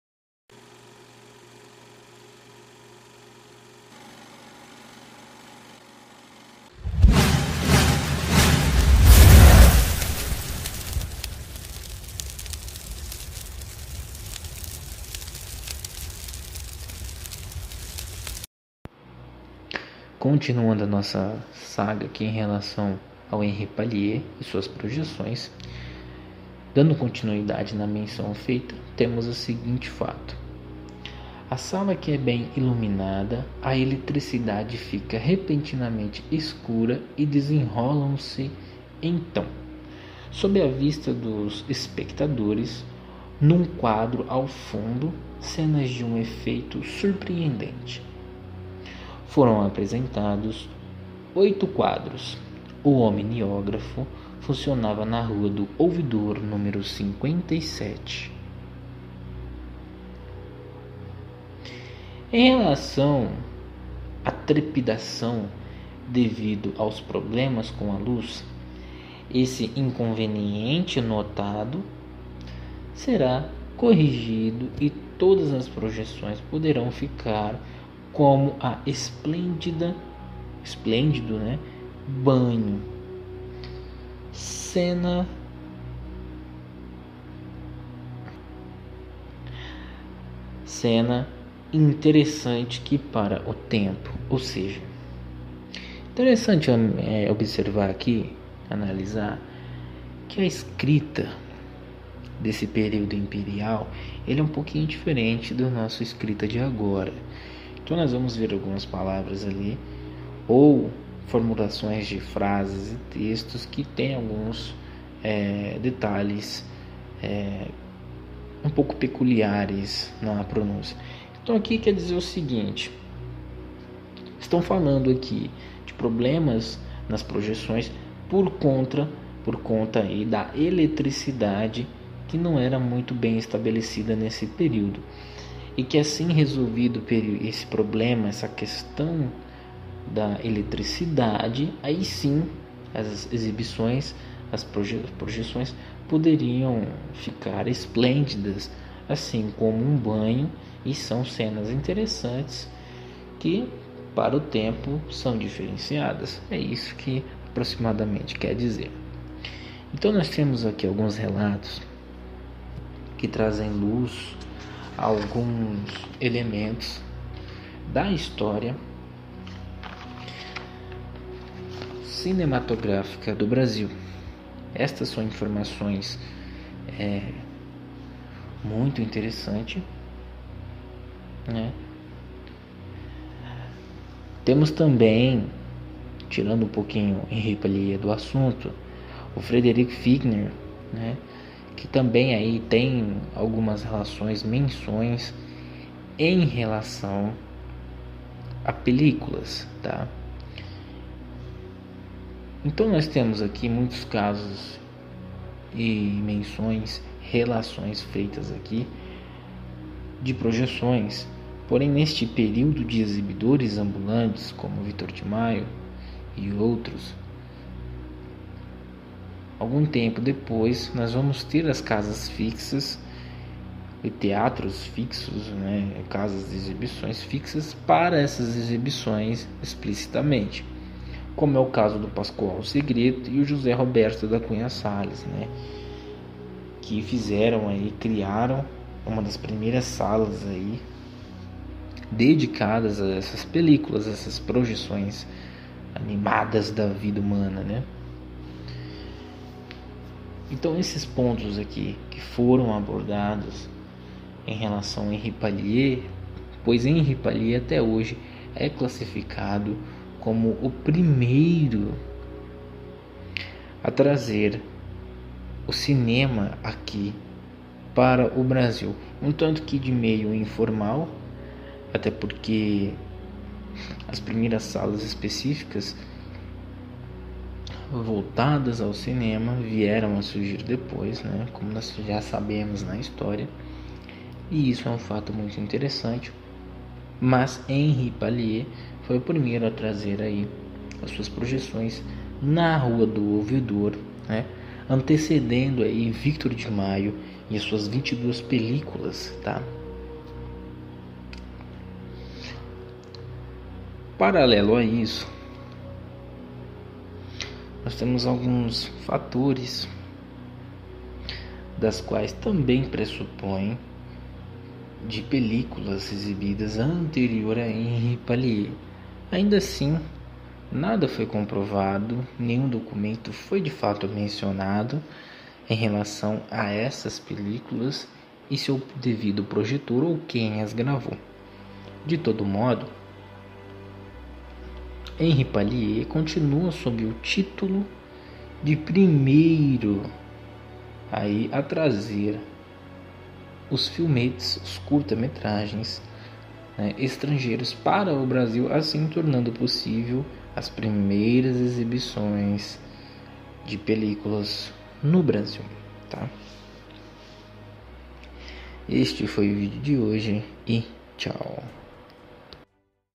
Henri Paillie 3
Henri Paillie foi o primeiro a projetar peliculas no brasil. Estes eventos foram constatados como "Omniographo" pelos jornais da época.